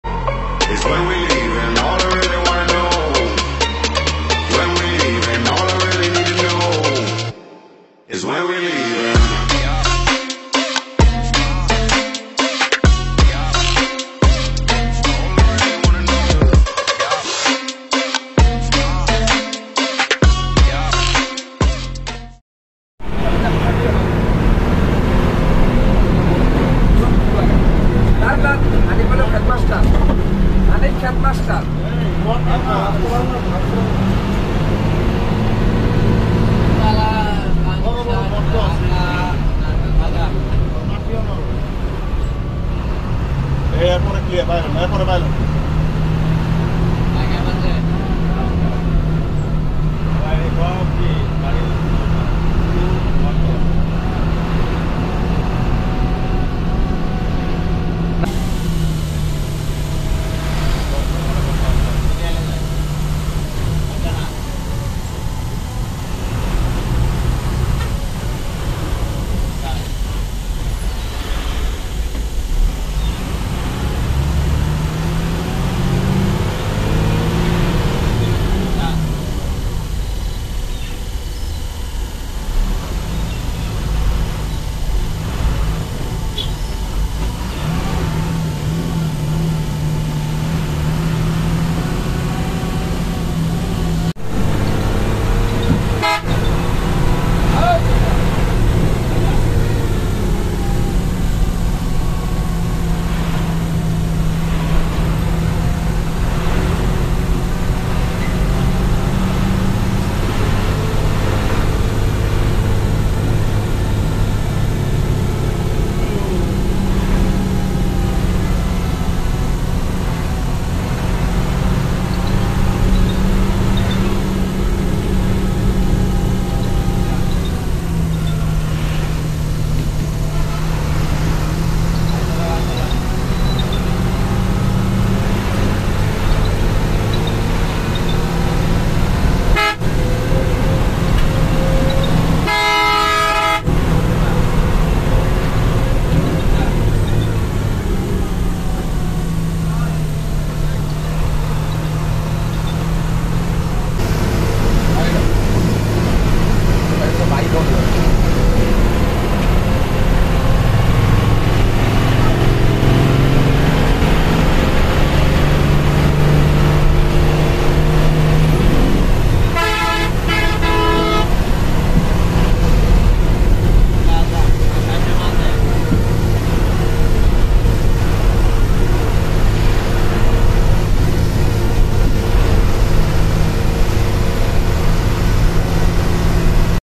HANIF VS JAMUNA TRAVELS!!🔥🔥তাউরাশ হানিফ এর টান এ অসহায় যমুনা ট্রাভেলস!!😳
এই ভিডিওটি একটি জার্নি ভিডিও হিসেবে গ্রহণ করা হয়েছে।
বিঃদ্রঃ গাড়িটি ট্যুর এর গাড়ি ছিলো এবং ড্রাইভার তার ১০০% দক্ষতা দিয়ে চালিয়েছেন।

ভিডিওটি ভালো লাগলে লাইক কমেন্ট শেয়ার করবেন।